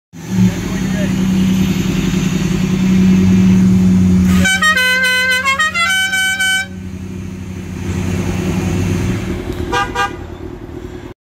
Dukes of Hazzard car
This cool car we saw at an auto body shop in Spartanburg SC